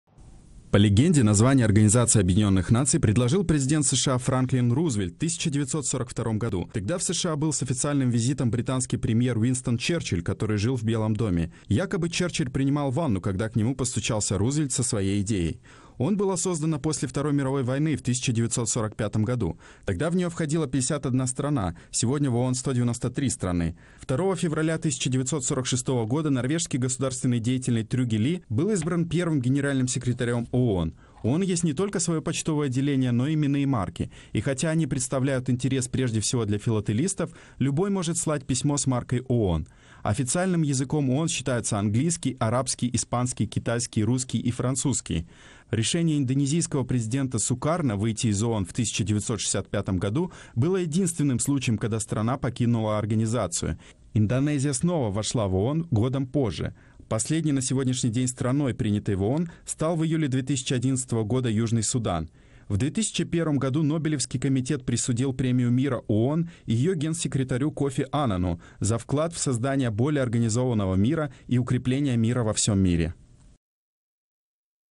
ООН – 71 год
На 26-й сессии в 1971 году Генассамблея провозгласила 24 октября международным праздником и предложила всем государствам отмечать его